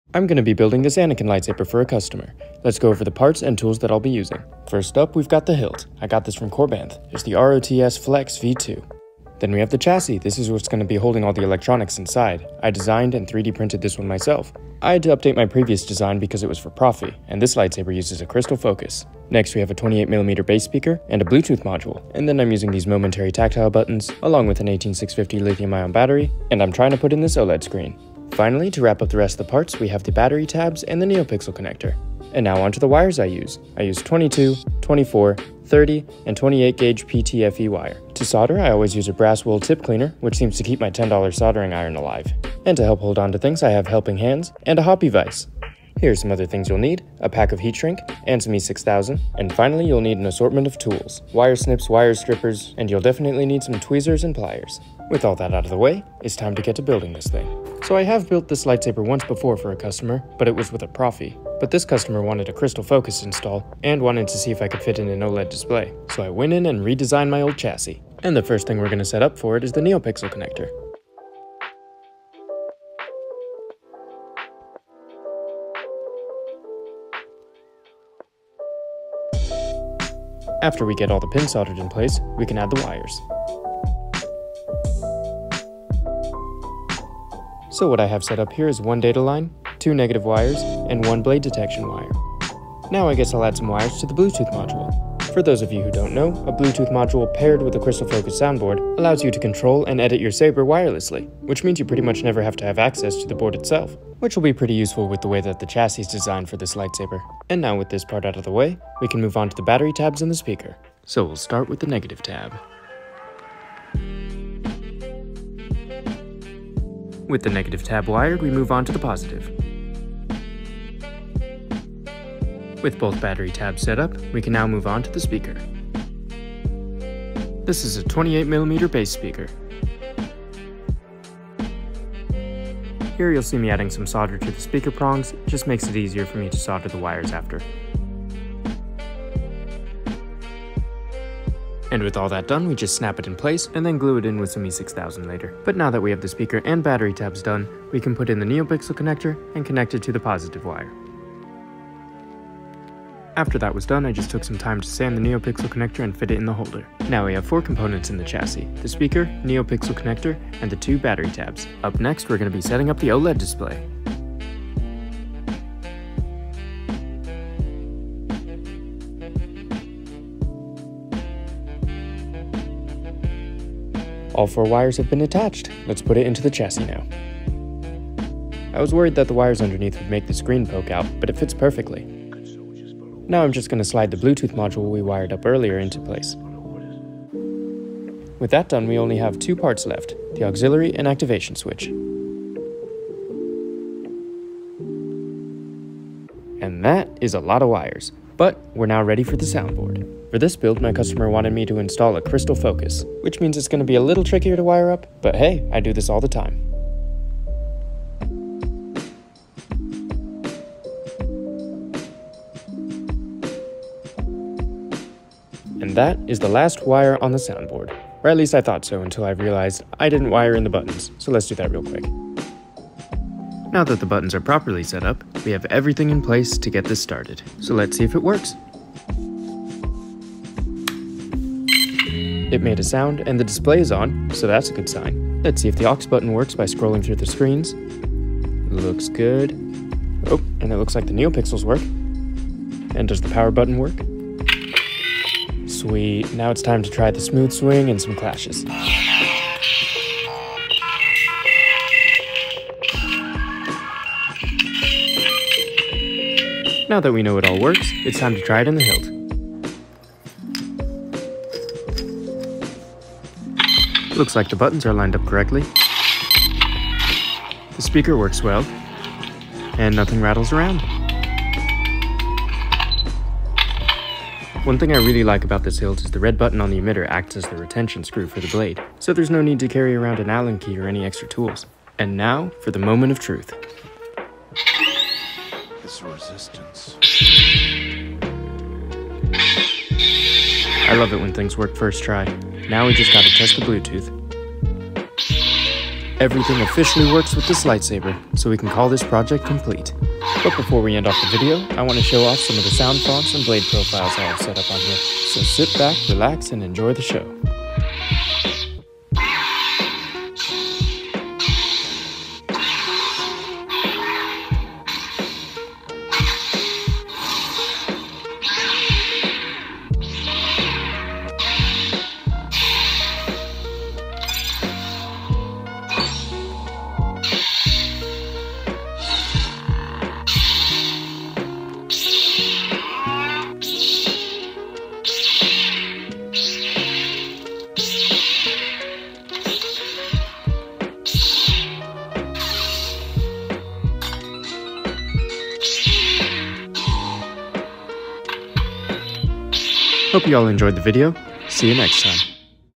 Making Anakin’s ROTS Lightsaber | CFX, Bluetooth, OLED Screen
Starting off with a quick overview of the parts and tools I used, I spent this week’s build video on making Anakin’s ROTS Lightsaber! This was a customer commission, and he wanted a CFX install with bluetooth and an oled display. So I designed and 3D printed my own chassis for it, and this is how the install went.

Music in this video is used with the permission of Closed On Sunday: